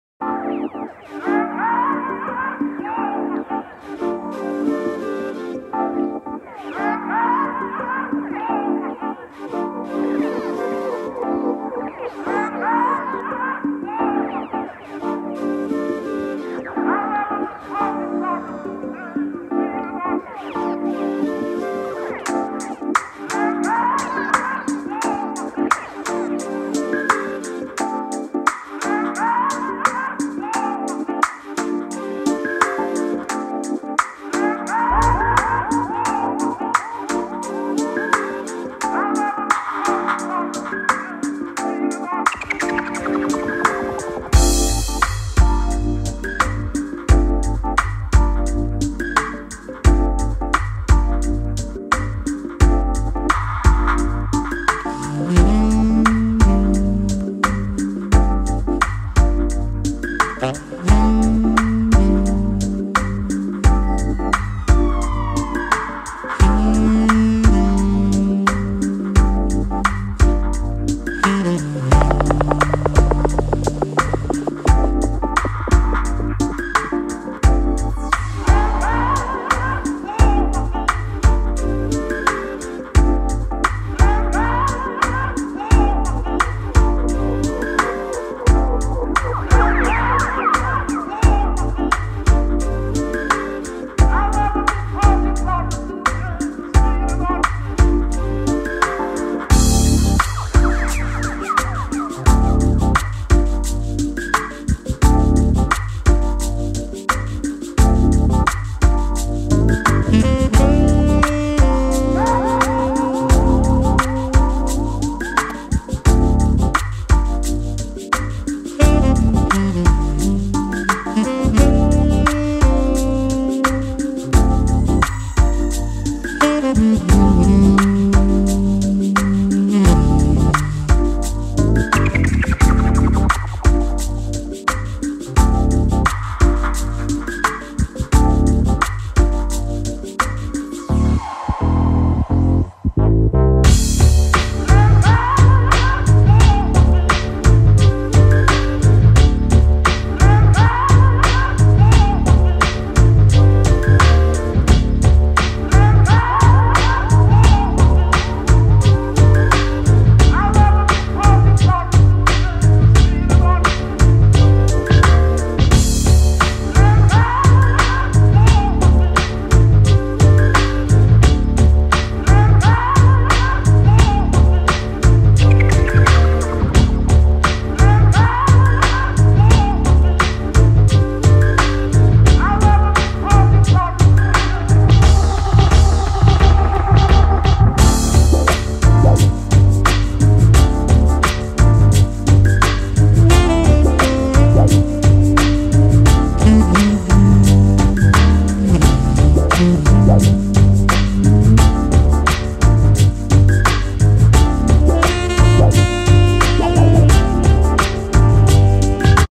Souq Marbarakia, Kuwait
Souq is an Arab term for market. If you want to have traditional experience while staying in Kuwait, come visit Souq Marbarakia. It's busy, vibrant and quite friendly :-)
Artist: Praful